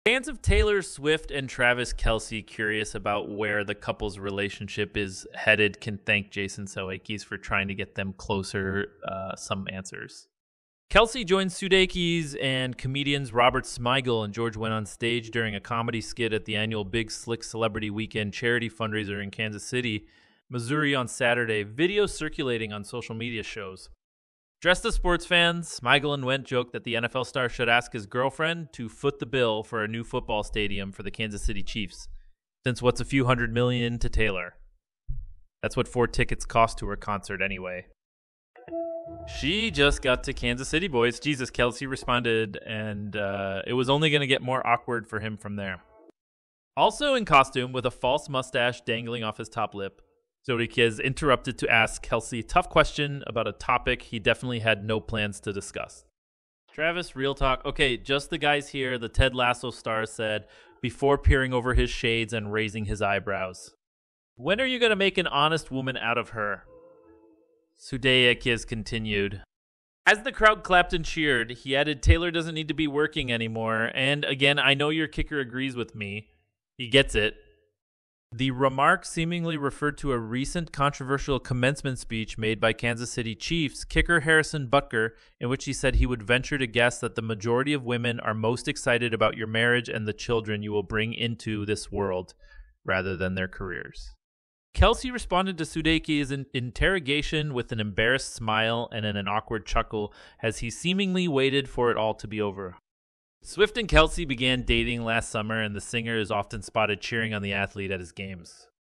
Travis Kelce is speechless as Jason Sudeikis pops awkward question about Taylor Swift
Fans of Taylor Swift and Travis Kelce curious about where the couple’s relationship is headed can thank Jason Sudeikis for trying to get them closer to some answers.

Kelce joined Sudeikis and comedians Robert Smigel and George Wendt onstage during a comedy skit at the annual Big Slick Celebrity Weekend charity fundraiser in Kansas City, Missouri on Saturday, video circulating on social media shows.